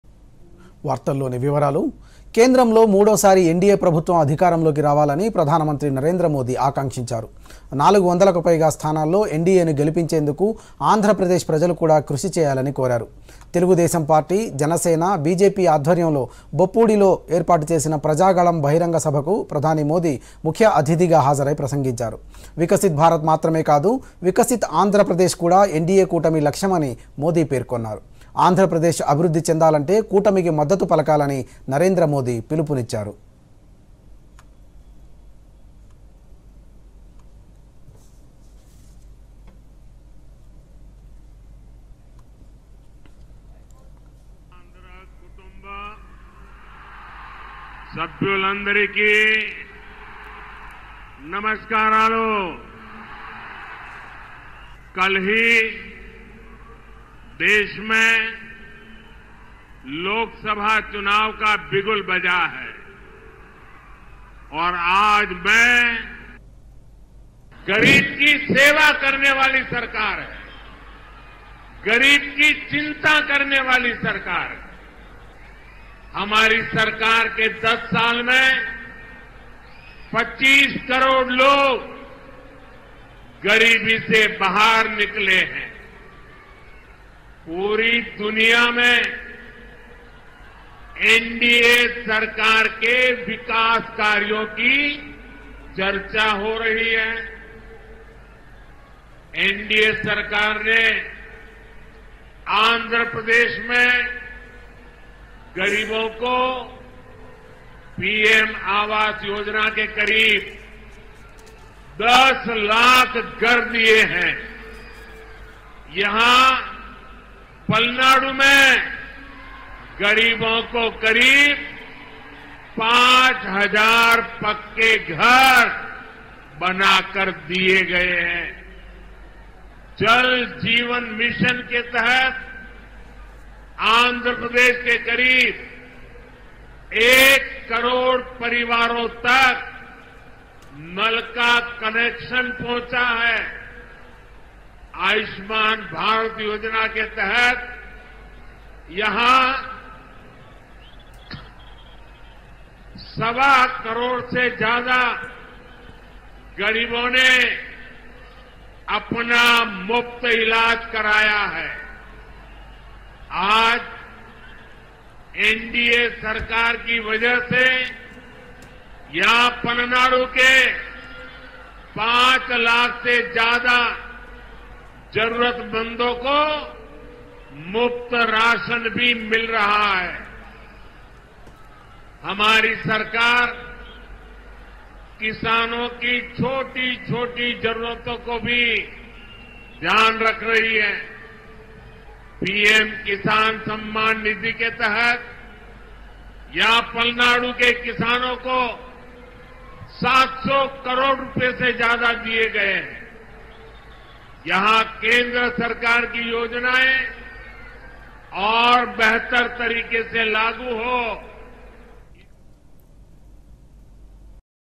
వికసిత్ భారత్ ఎన్డీయే కూటమి లక్ష్యమన్ననరేంద్ర మోదీ | PM Modi at Andhra Pradesh | 🟥DD News Telangana
కేంద్రంలో మూడోసారి ఎన్డీయే ప్రభుత్వం అధికారంలోకి రావాలని ప్రధానమంత్రి నరేంద్ర మోదీ ఆకాంక్షించారు. 400 పైగా స్థానాల్లో ఎన్డీయేను గెలిపించేందుకు ఆంధ్రప్రదేశ్ ప్రజలకు కూడా కృషి చేయాలని కోరారు.  తెలుగుదేశం పార్టీ, జనసేన, బీజేపీ ఆధ్వర్యంలో బొప్పూడిలో ఏర్పాటు చేసిన ప్రజాగళం బహిరంగ సభకు ప్రధాని మోదీ ముఖ్య అతిథిగా హాజరై ప్రసంగించారు. వికసిత్ భారత్ మాత్రమే కాదు.. వికసిత్ ఆంధ్రప్రదేశ్ కూడా ఎన్డీయే కూటమి లక్ష్యమని మోదీ పేర్కొన్నారు. ఆంధ్రప్రదేశ్ అభివృద్ధి చెందాలంటే కూటమికి మద్దతు పలకాలని నరేంద్ర మోదీ పిలుపునిచ్చారు.